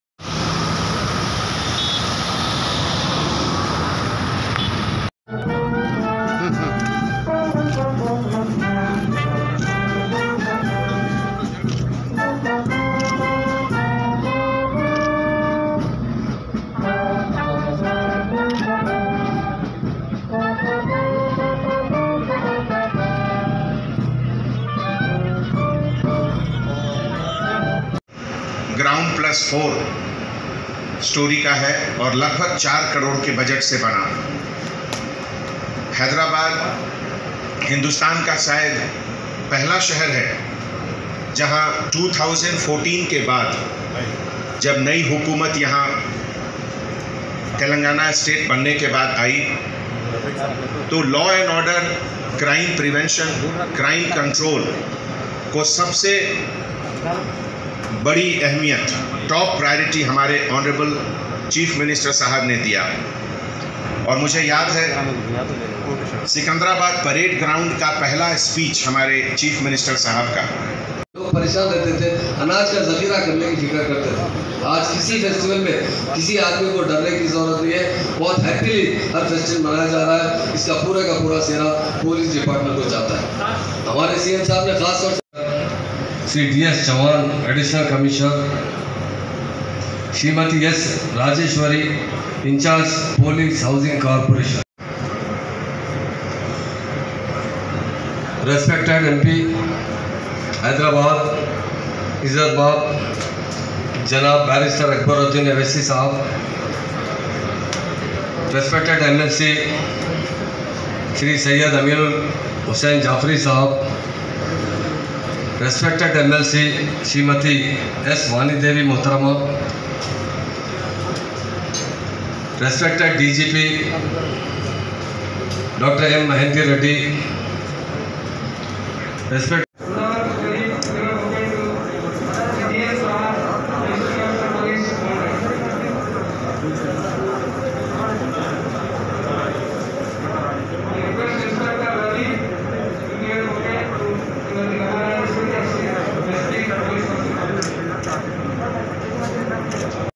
AAJ CHANDRAYANGUTTA POLICE STATION KA IFTATAH JANAB H. M MAHMOOD ALI AUR M. P HYD
AAJ CHANDRAYANGUTTA POLICE STATION KA IFTATAH JANAB H. M MAHMOOD ALI AUR M. P HYD BARRSITER ASAD UDDIN OWASI KAY HATOU AMAL MAI AYAA YA POLICE SATIOTON 15OO . SQ G+4 BULDING HAI IS KO 4 CRORE KI LAGAT SE TAYAR KIYA GAYA HAI IS MUQAYA PAR . DGP. TELANGANA MAHINDER REDDY . C. P. HYD ANJANI KUMAR . ADDL. CP. ANILKUMAR . MLC. AMEEN UL HASSAN JAFFER MLC SURBHA VANI DEVI AUR TAMAM CORPOEATERS MUJOOD TGA